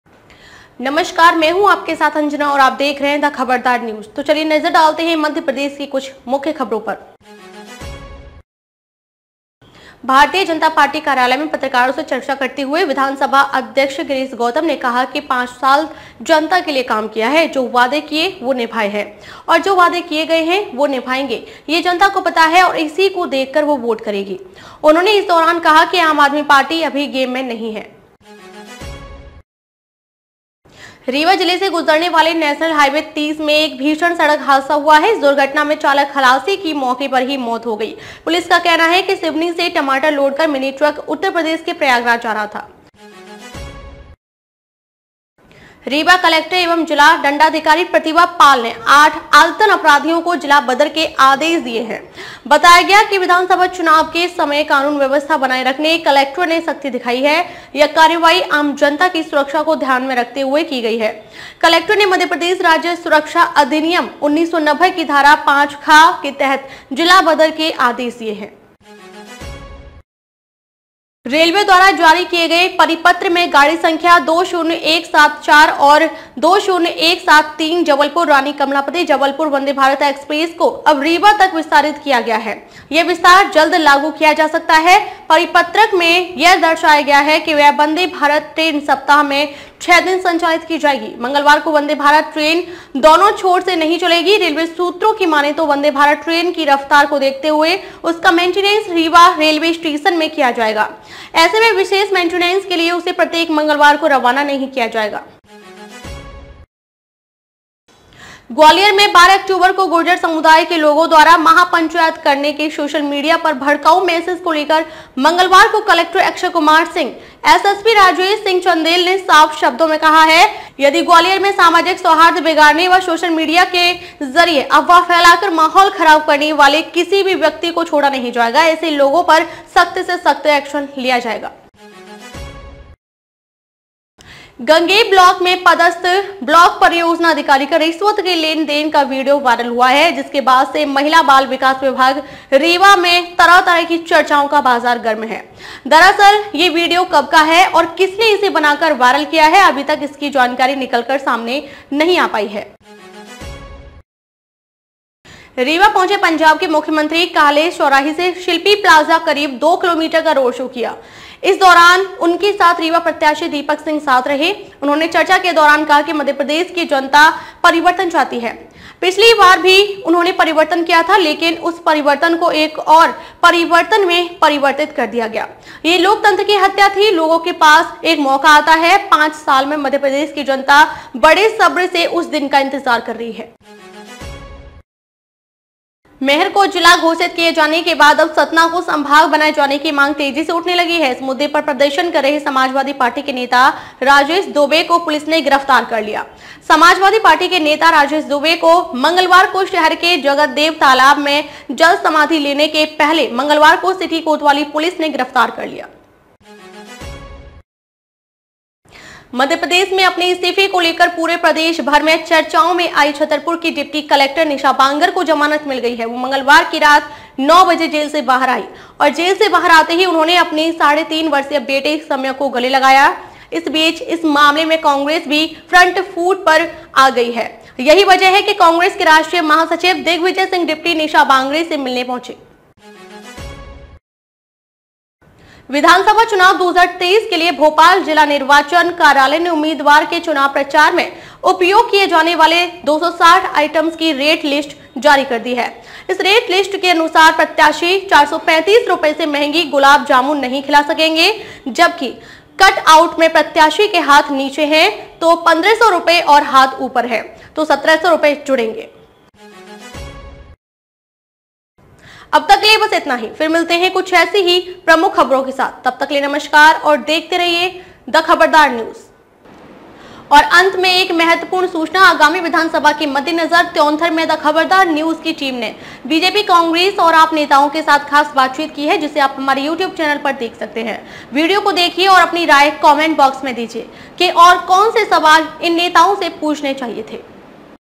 Today's TOP Headlines। आज की सुर्खियां । Rewa News। MP Latest News.
MP TOP !) News: भारतीय जनता पार्टी कार्यालय में पत्रकारों से चर्चा करते हुए विधानसभा अध्यक्ष गिरीश गौतम ने कहा की पांच साल जनता के लिए काम किया है जो वादे किए वो निभाए है और जो वादे किए गए है वो निभाएंगे ये जनता को पता है और इसी को देखकर वो वोट करेगी,,, उन्होंने इस दौरान कहा की आम आदमी पार्टी अभी गेम में नही है.
 रीवा जिले से गुजरने वाले नेशनल हाईवे-30 में एक भीषण सड़क हादसा हुआ है। इस दुर्घटना में चालक-खलासी की मौके पर ही मौत हो गई। पुलिस का कहना है कि सिवनी से टमाटर लोड कर मिनी ट्रक उत्तर प्रदेश के प्रयागराज जा रहा था ।
रीवा कलेक्टर एवं जिला दण्डाधिकारी प्रतिभा पाल ने 8 आदतन अपराधियों को जिला बदर के आदेश दिए हैं। बताया गया कि विधानसभा चुनाव के समय कानून-व्यवस्था बनाए रखने कलेक्टर ने सख्ती दिखाई है। यह कार्यवाही आमजनता की सुरक्षा को ध्यान में रखते हुए की गई है। कलेक्टर ने मध्यप्रदेश राज्य सुरक्षा अधिनियम 1990 की धारा 5 ख के तहत जिला बदर के आदेश दिए हैं।